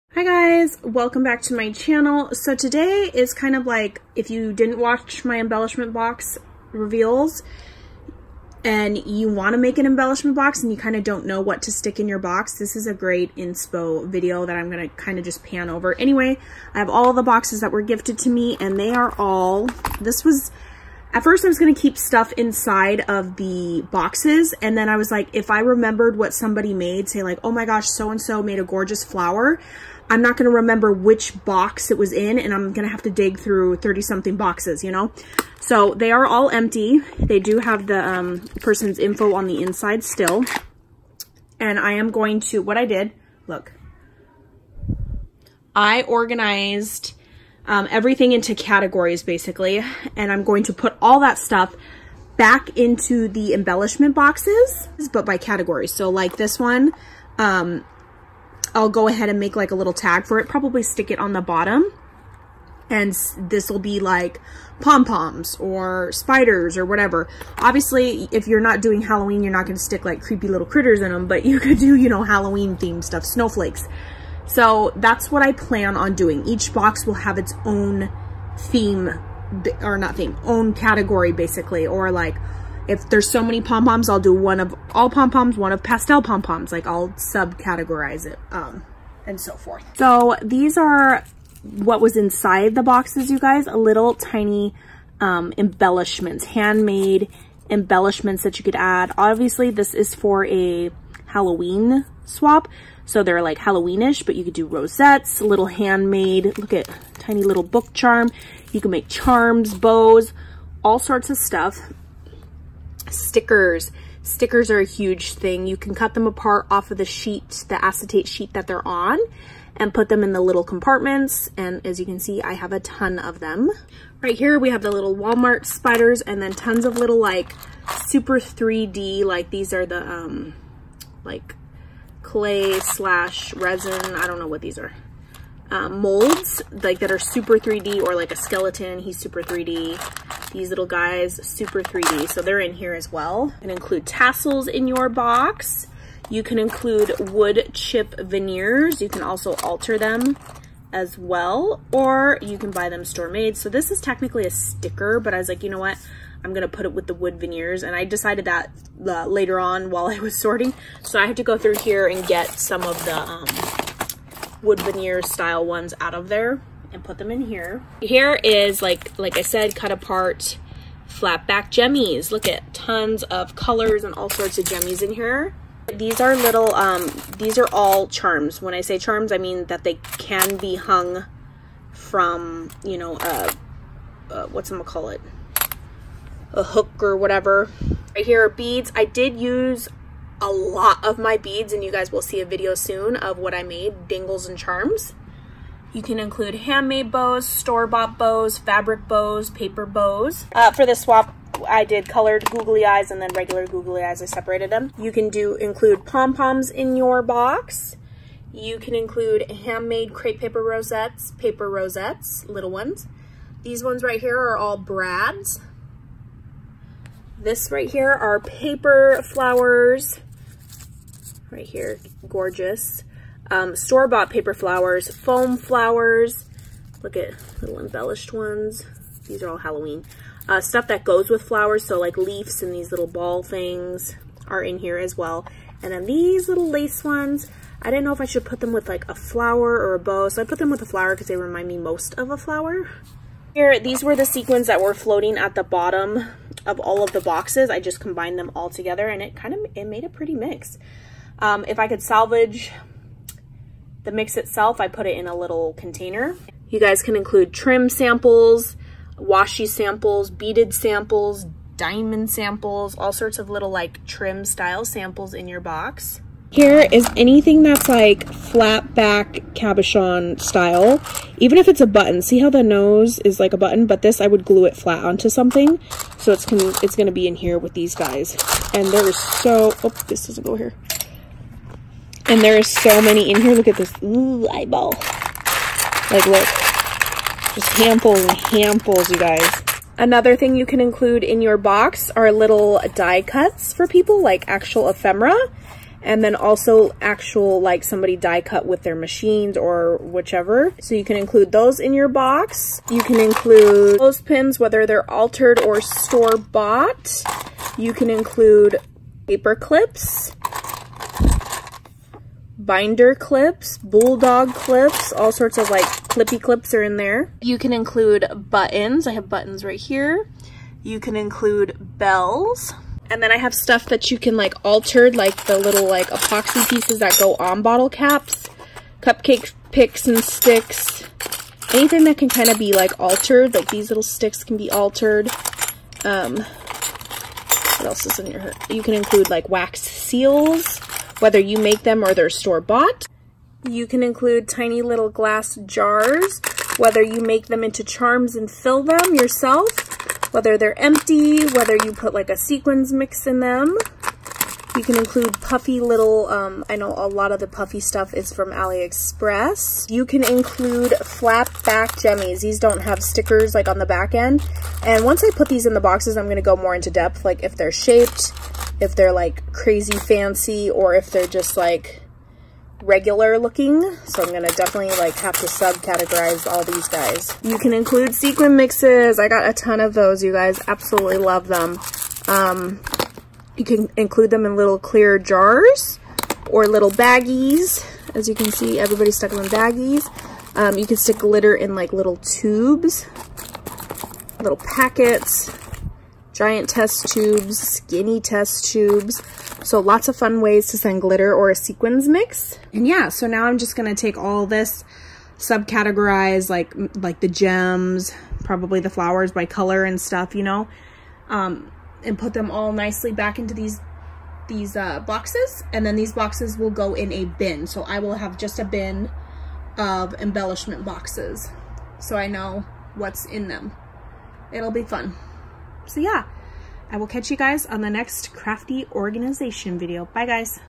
Embellishment Box IDEAS! What to stick inside??
received all this yummyness from my embellishment box swap I hosted.
xoxo
Toni